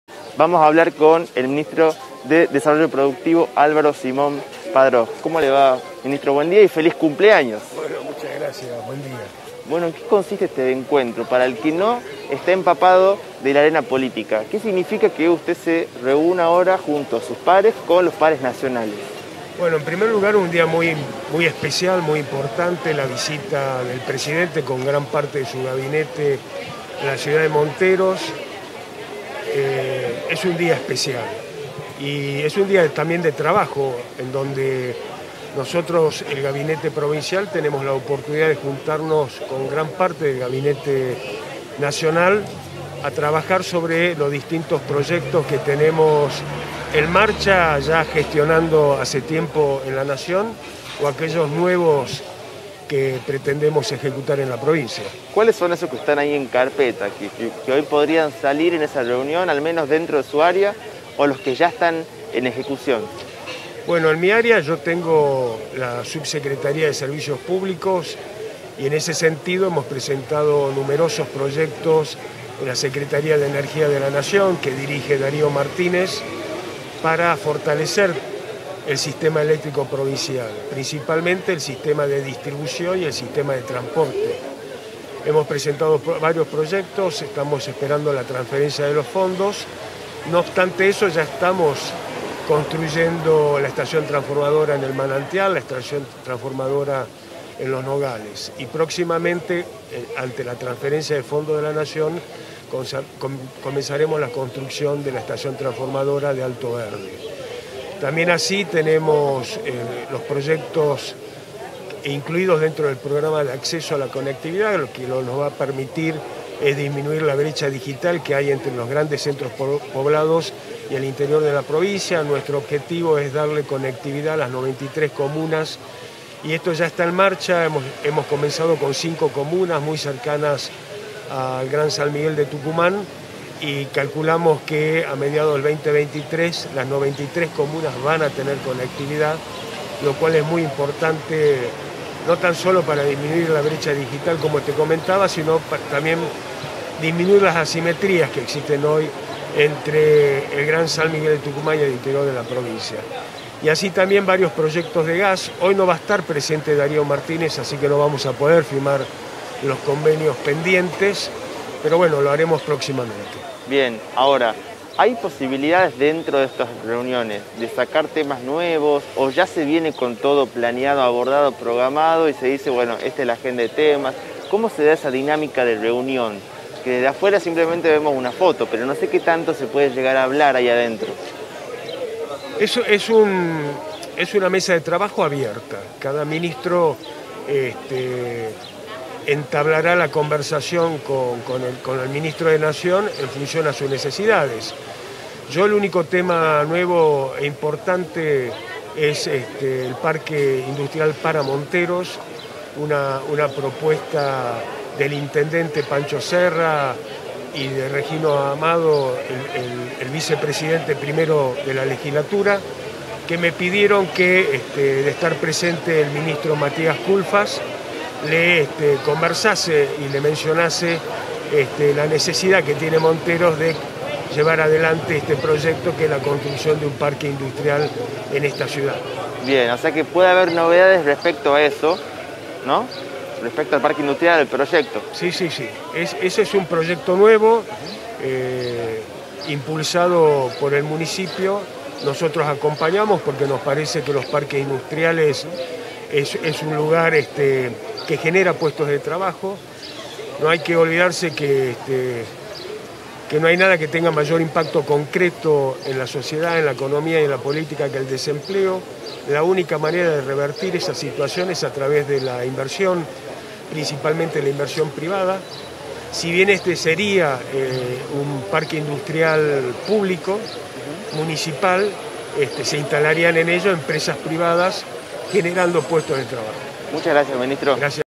Visita presidencial: "queremos mencionar la necesidad de llevar adelante un parque industrial"
Entrevista a Álvaro Simón Padrós, ministro de desarrollo productivo